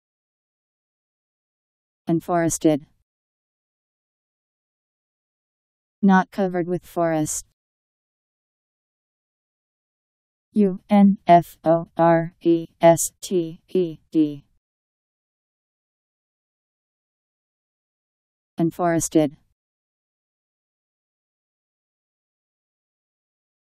Unforested Meaning
Video shows what unforested means. Not covered with forest..  Unforested Meaning. How to pronounce, definition audio dictionary. How to say unforested. Powered by MaryTTS, Wiktionary